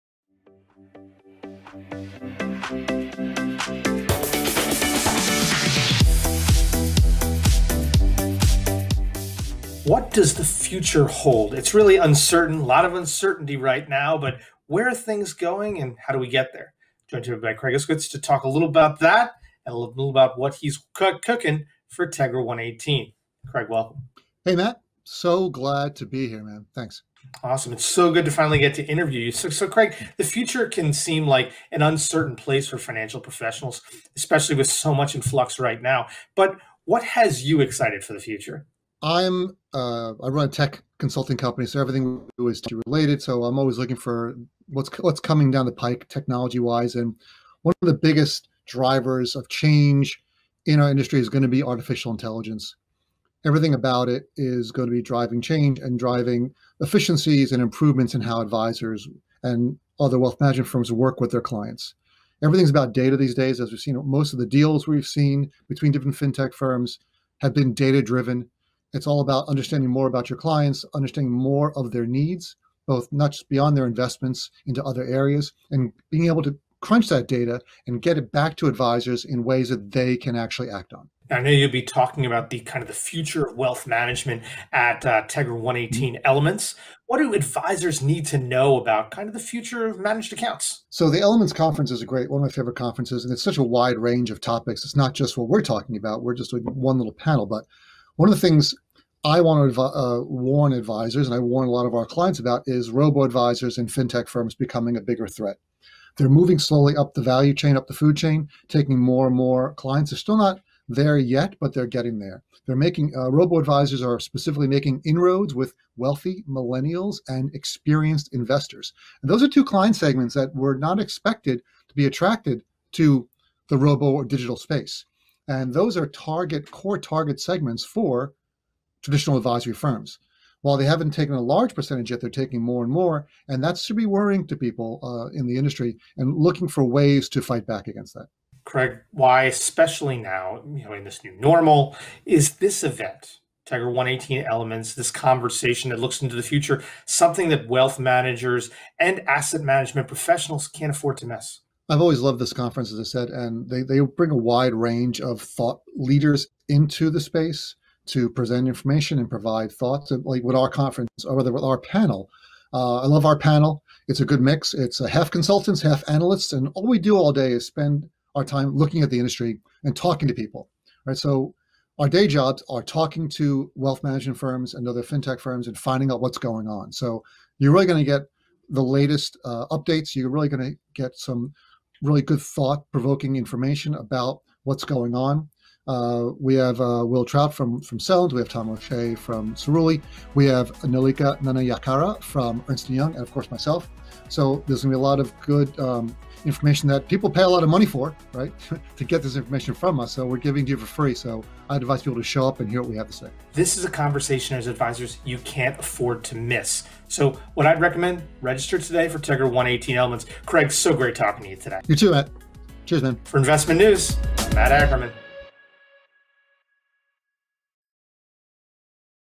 What Does The Future Hold
Craig Iskowitz from Ezra Group joins to share his thoughts on the future of technology, how AI will be the biggest driver of change, and why he loves the Tegra118 Elements event.